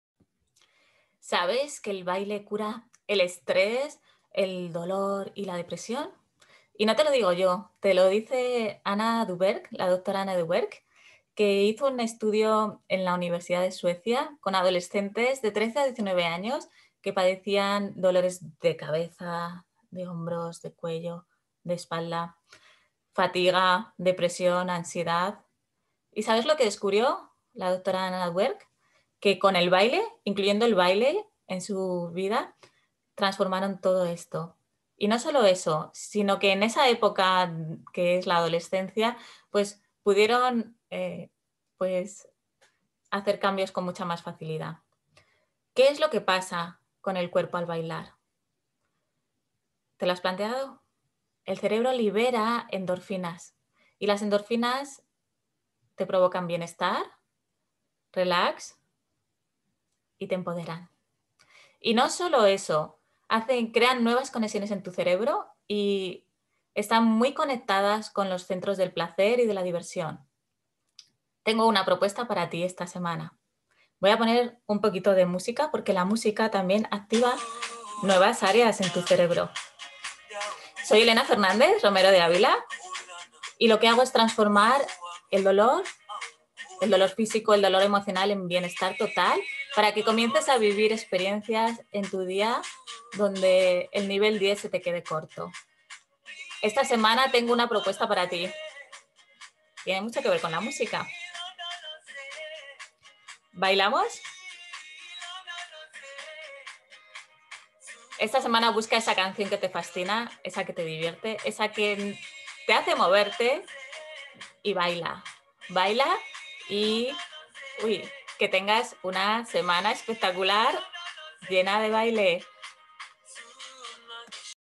Bailar el mejor analgésico para tu cuerpo
¿Qué pasa en el cuerpo al bailar?
Cuando bailamos, en nuestro cerebro se liberan endorfinas, unos neurotransmisores que generan una sensación de bienestar, relajación, alegría y empoderamiento. La música y el baile no solo activan en nuestro cerebro los circuitos motores y sensoriales sino también los centros del placer